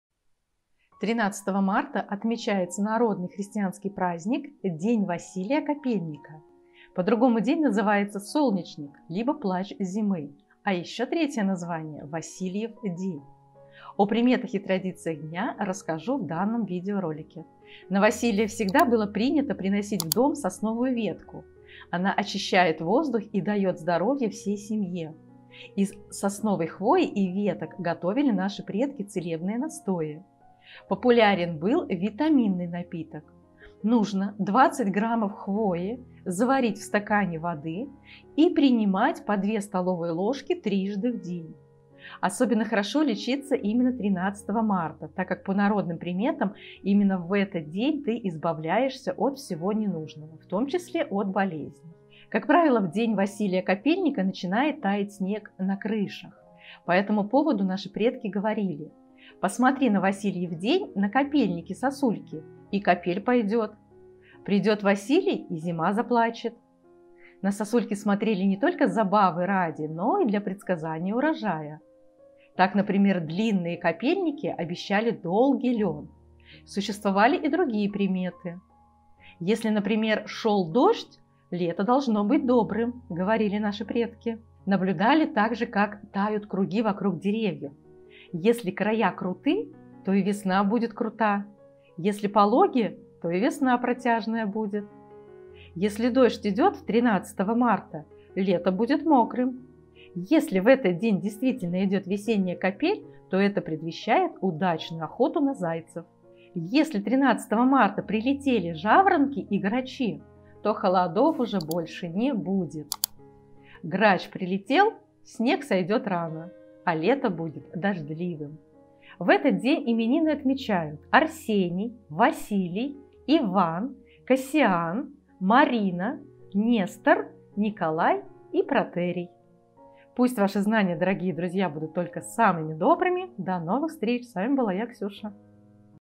13 марта - Васильев День, КАПЕЛЬНИК. Что нужно сделать сегодня: народные приметы по погоде
13 марта - Васильев День по народному календарю. В старину этот день считался отправной точкой в году, сроком найма и начала хозяйственных договоров. В этот день на Руси ходили по домам с пожеланиями добра, богатства и мира.
На Руси этот день называли "Василий Великий", "Васильев день", "Васильев вечер", "Василий – покровитель свиней", "Василий-свинятник", "свиной праздник".